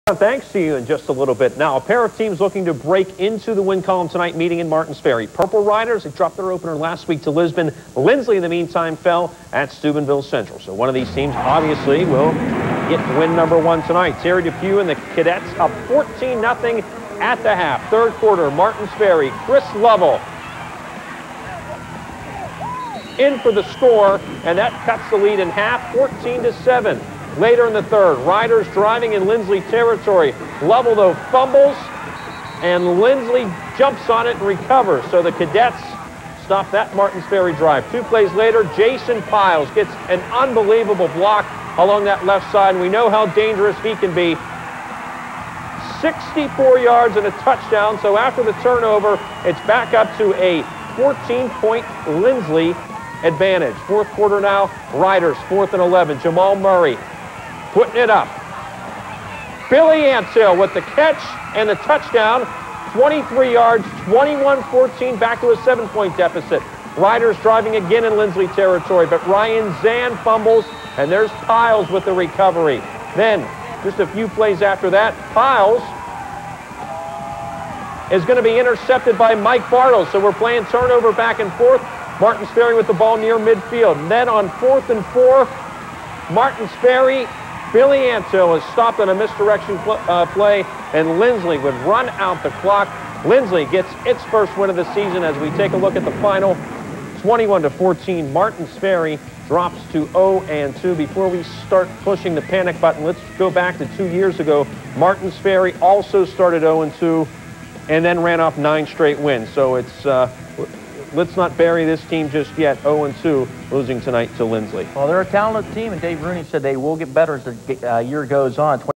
OVAC football - 2005 - Linsly v. Martins Ferry
Week 2 - September 2005 - Linsly Cadets v. Martins Ferry Purple Riders at Purple Rider Stadium, Martins Ferry, Ohio. Linsly knocked off the Riders.